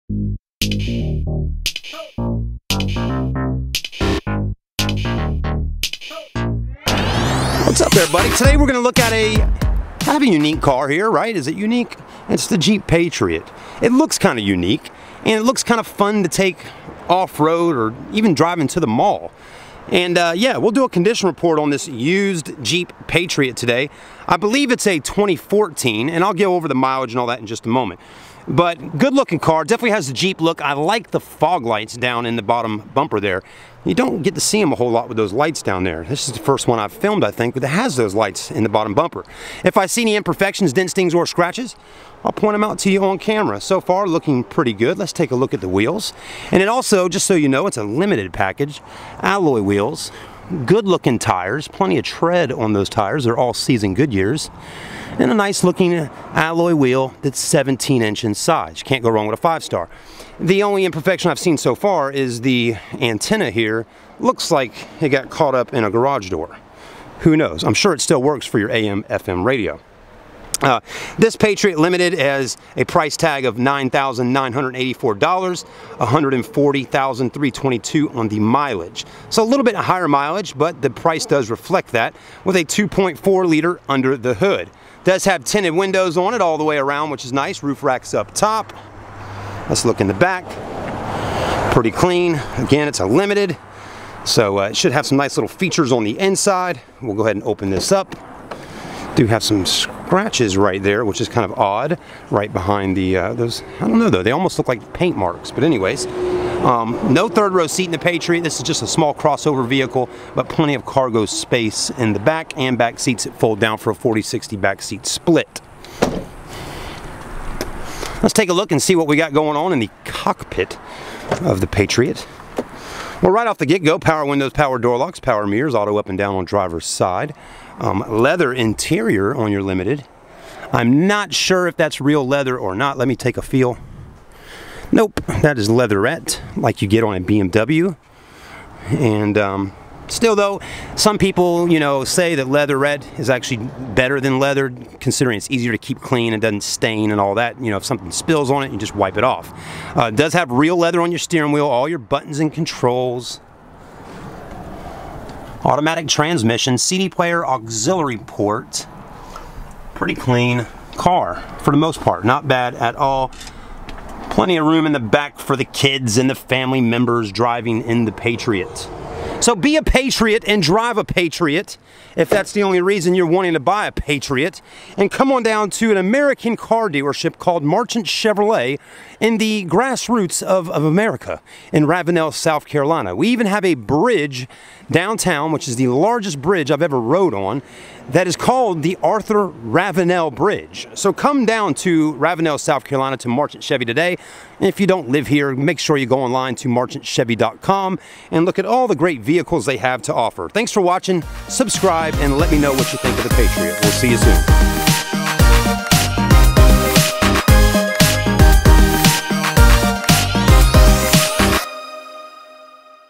2014 Jeep Patriot - For Sale Review & Condition Report at Marchant CHEVY - June 2017
I'm reviewing this Jeep Patriot and I'm feeling great while talking about it! Whats your thoughts on this vehicle? If your interested in test driving stop by or visit us online!